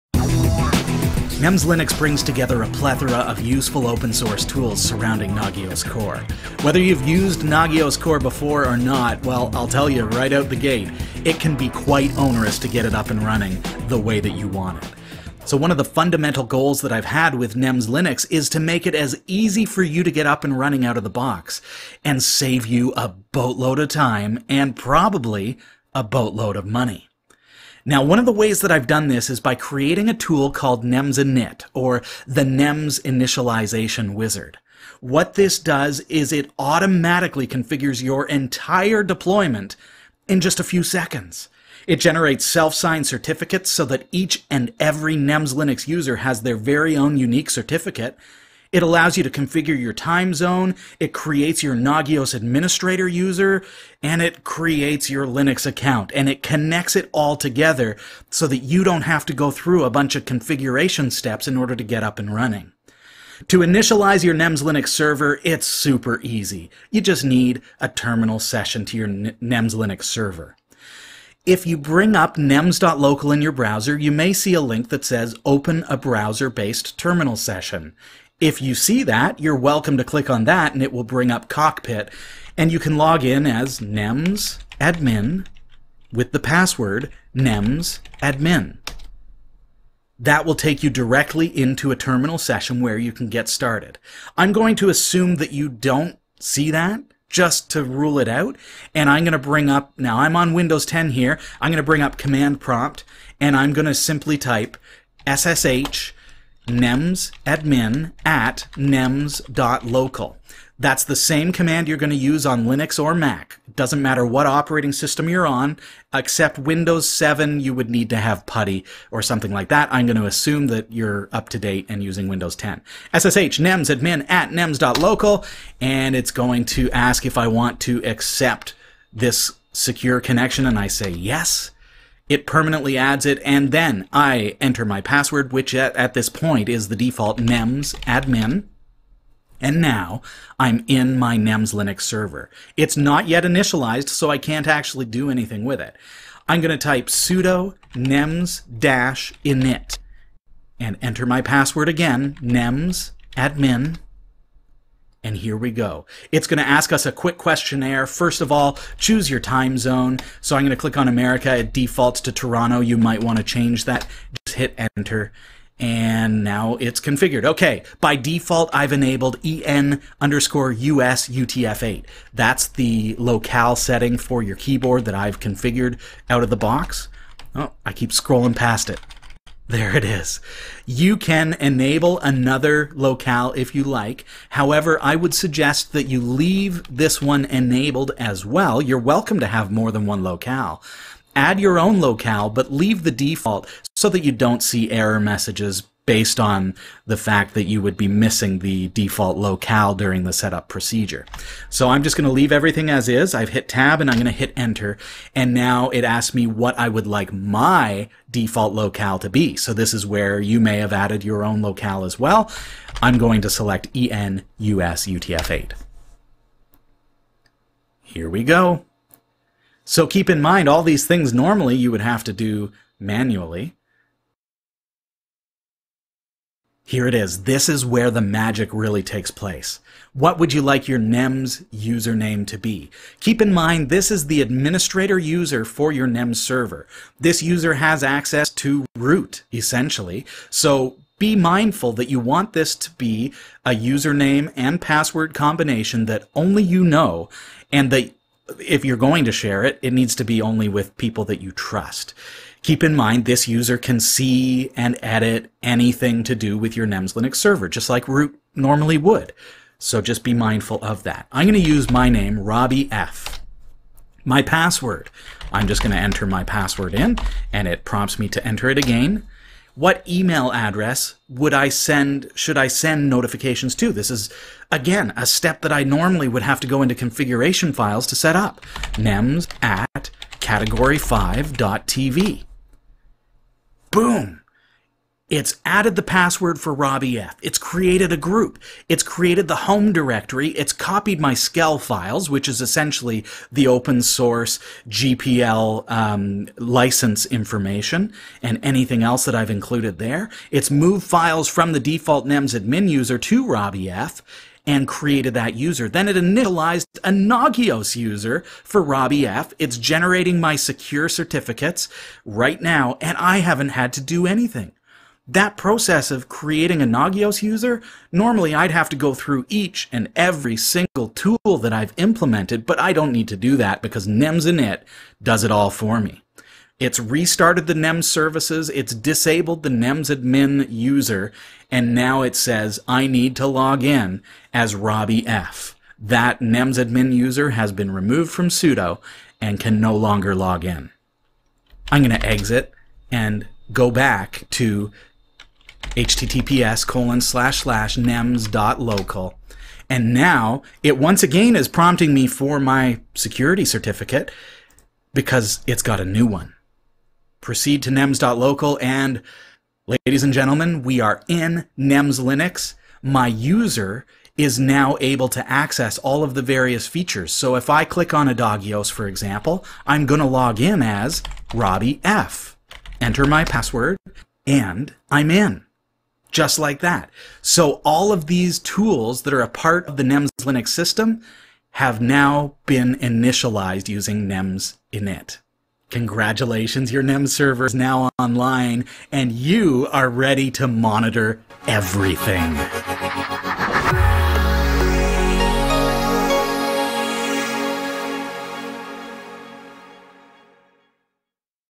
NEMS Linux Commands: nems-init
In order to begin using your new NEMS Server, you must first initialize it.

The nems-init process makes initialization quick and easy by automating many of the first-boot tasks for you. After initialization, you’ll be left with a fully-operational NEMS Server, complete with a small selection of sample check commands.